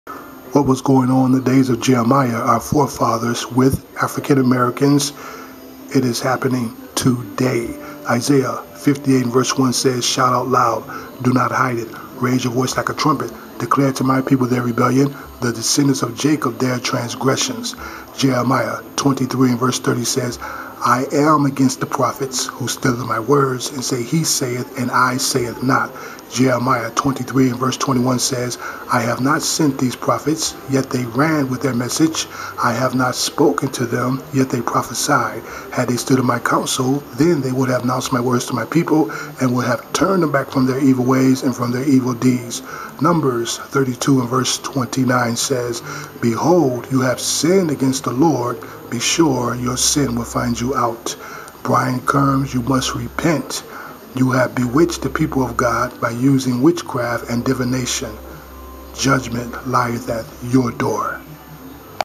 LEXI ALLEN GOES OFF ON BRIAN CARN!!! 3-16-2016
Repent Brian carn you have proven to be a false prophet before this psychic messed, was exposed about you.'' Matthew7:15